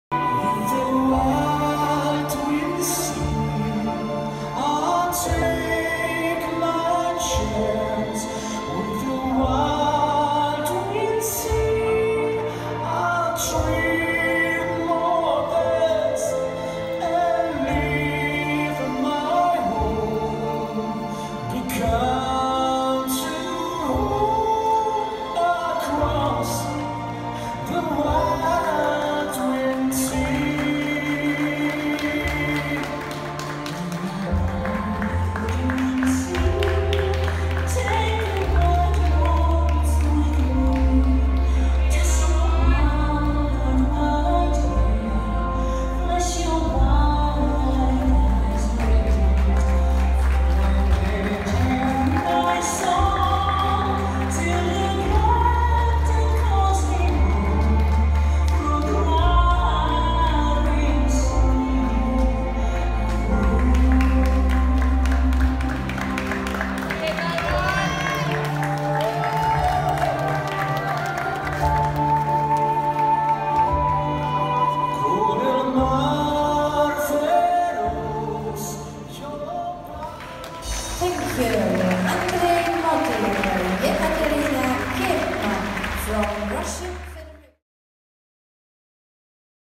Andrey Motyl - Ekaterina Kim, RUS, Final Solo English Waltz
PRAGUE
15TH - 17TH OF SEPTEMBER 2017
CZECH REPUBLIC OPEN CHAMPIONSHIPS